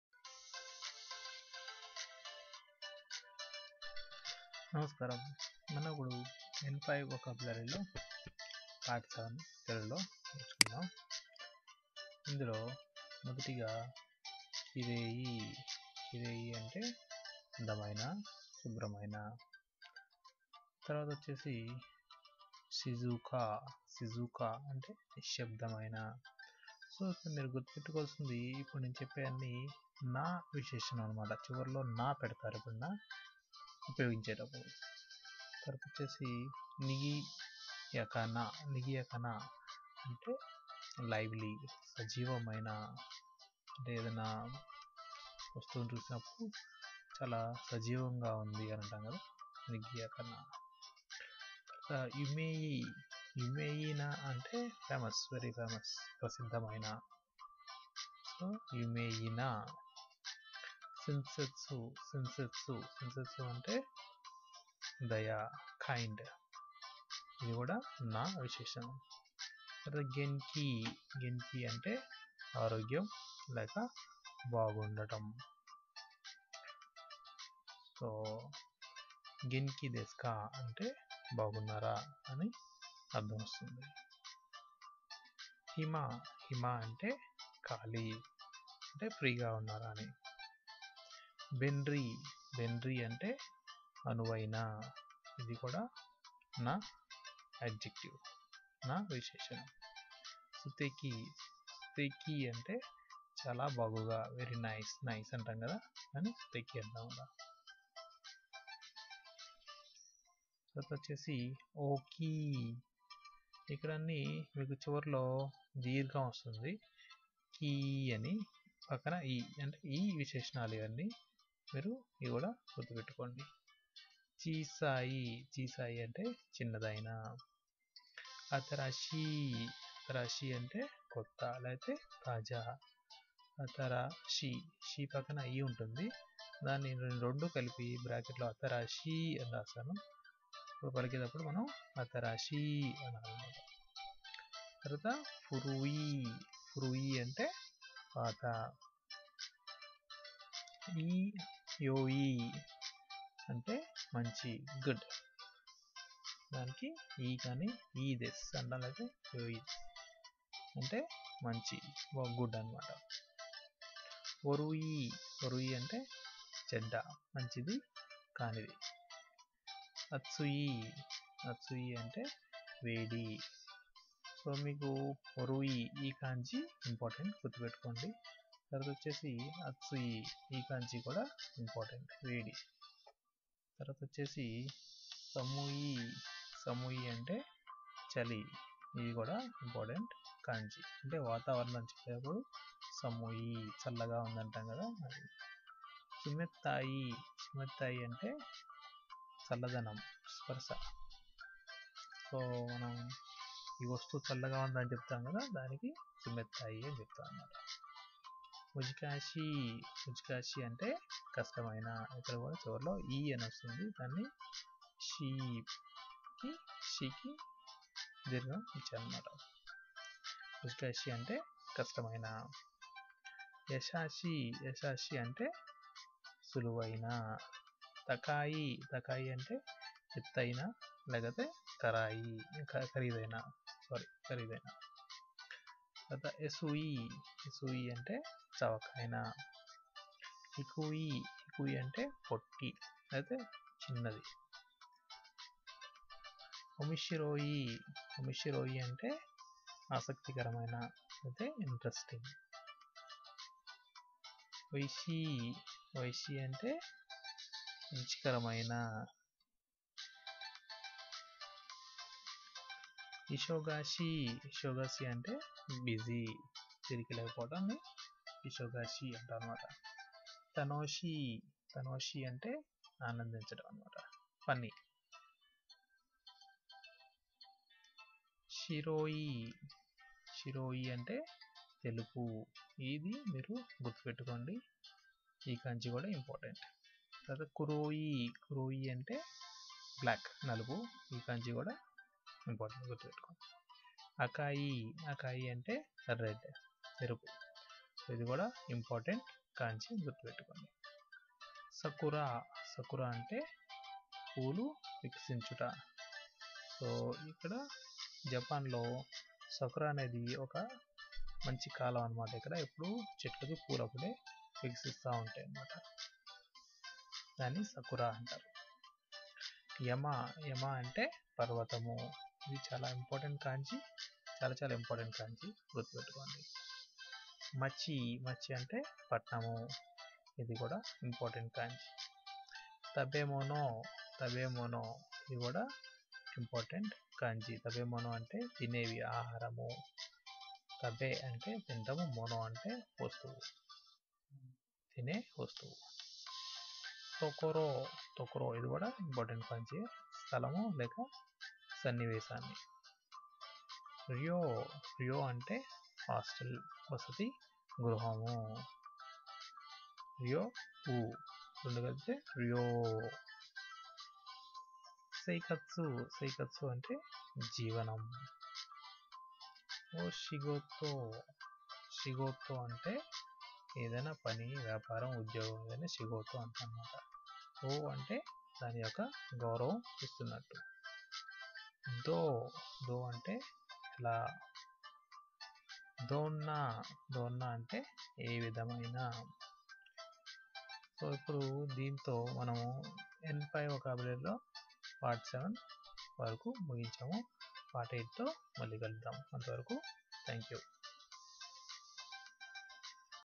Japanese N5 vocabulary part 7 Telugu
This video is Japanese vocabulary in Telugu and learn Japanese with easy and make use of it. This vocabulary allows you to learn Japanese words which are used in daily life